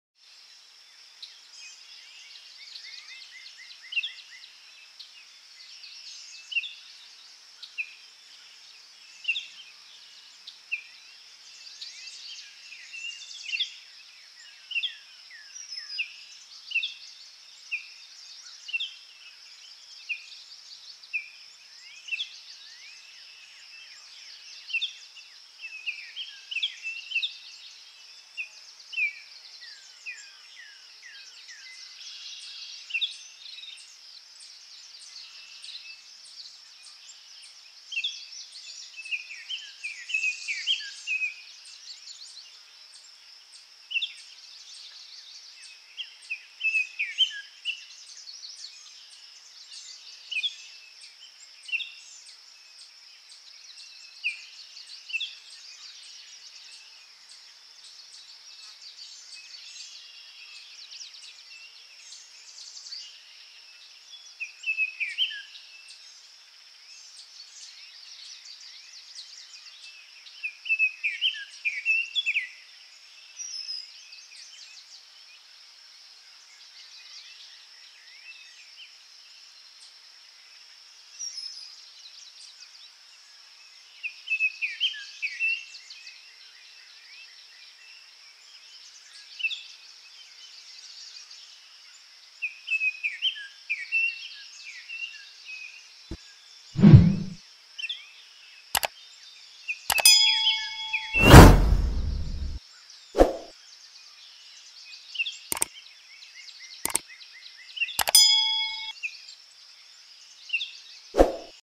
Senegal thick knee bird (Pench Tiger reserve) (สำรองเพนช์ ไทเกอร์) (પેંચ ટાઈગર રિઝર્વ)
Senegal thick-knees are medium-large waders with strong black and yellow black bills, large yellow eyes — which give them a reptilian appearance — and cryptic plumage. They are similar but slightly smaller than the Eurasian stone-curlew, which winters in Africa. The long dark bill, single black bar on the folded wing, and darker cheek stripe are distinctions from the European species. Senegal thick-knee is striking in flight, with a broad white wing bar.
This species has a preference for dry open habitats with some bare ground, preferably near water. It lays two blotchy light brown eggs on a ground scrape. It is most active at dawn and dusk. The song is a loud pi-pi-pi-pi-pi-pi-pi.  Food is insects, crustaceans and other invertebrates. It will also take other small prey.
#(पेंच व्याघ्र प्रकल्प)